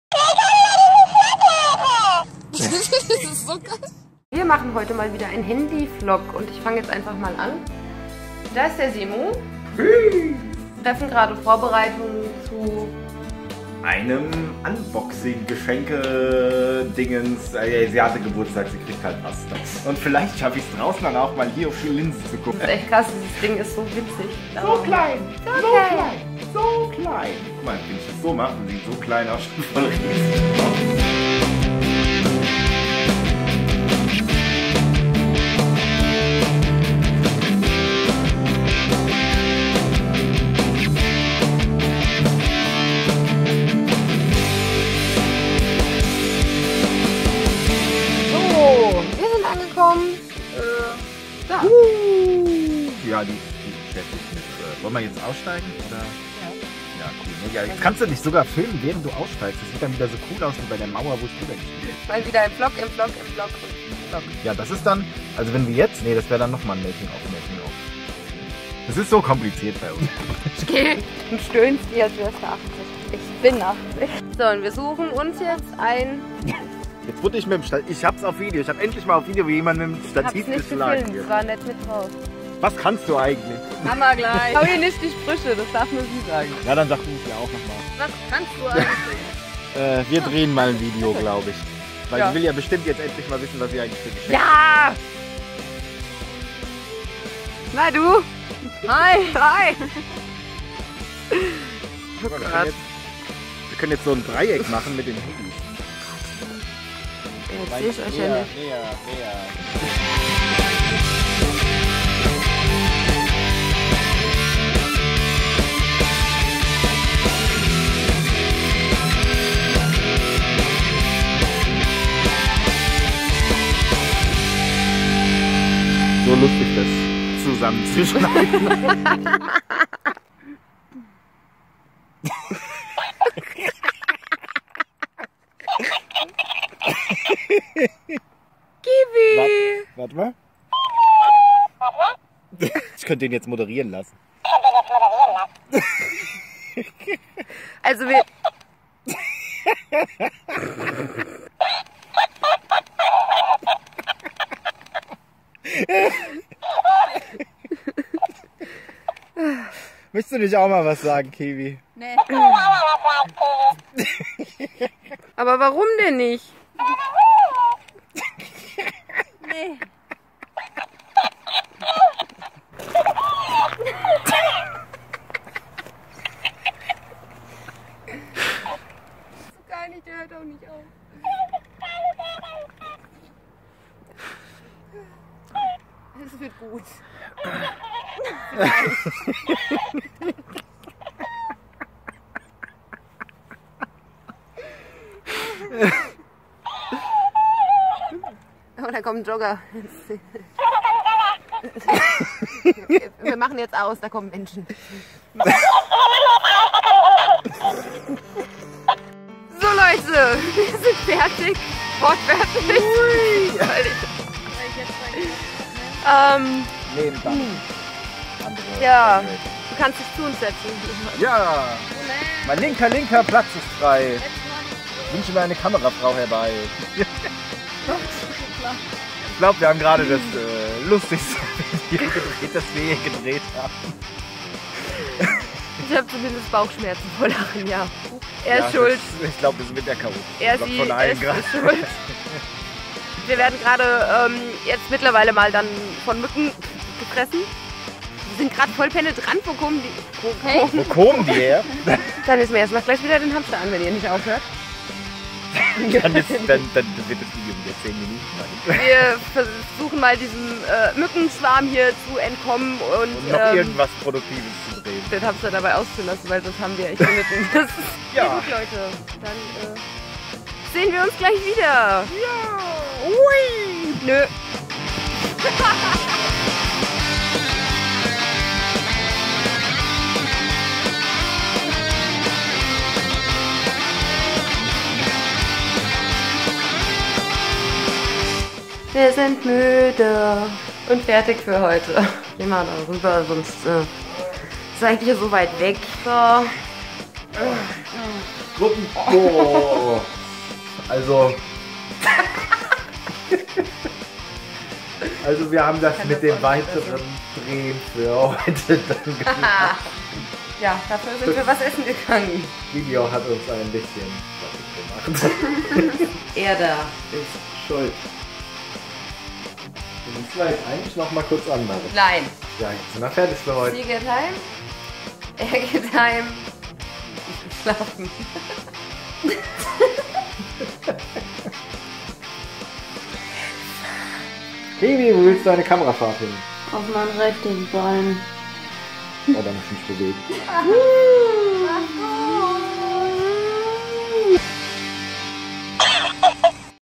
▓ YOUTUBE Videos drehen - Der GUTE LAUNE Hamster VLOG ▓ "Making of" VLOG auf deutsch
Youtube Videos drehen ...Wie ein Plüschtier unseren Drehtag sprengte - wir vloggen uns durch den Drehtag zum Hamstervideo und zeigen in diesem Making of VLOG wie auch Kiwi ihren Lachflash hatte :D Diesmal als Multi Kamera VLOG - Sprich: jeder hat mal sein Smartphone gezückt und ich schnippelte von Handy zu Handy ^^ / deutsch

Die komplette Musik im Video ist wie immer selber geschrieben & produziert. Ihr findet die Songs alle auch inklusive Musikvideo auf meinem Youtubekanal!

Music produced in Darmstadt & Odenwald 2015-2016 by Semu